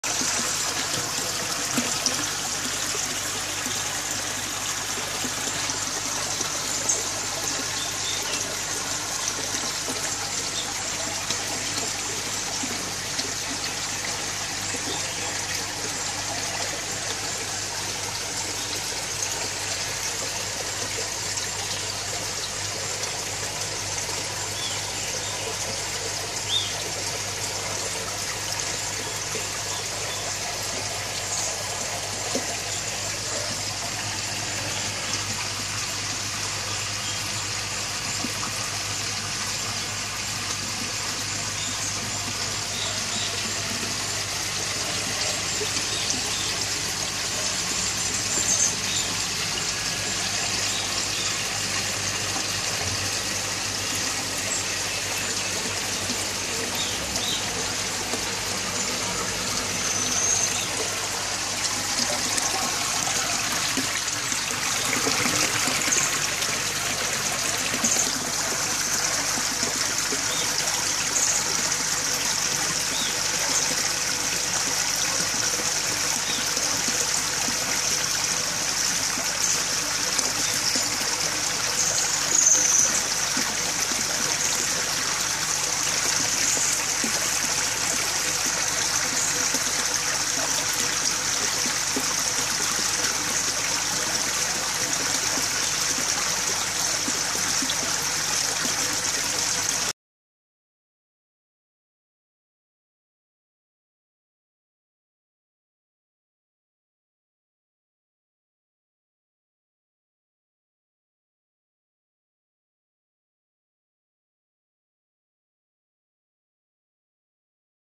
New Chambers filtration with K1 moving bed filter, December 2014
The Chamber Filters, in different volume capacities, are being made and commercialized in Mauritius by Asvin Bahadur and Aquatic.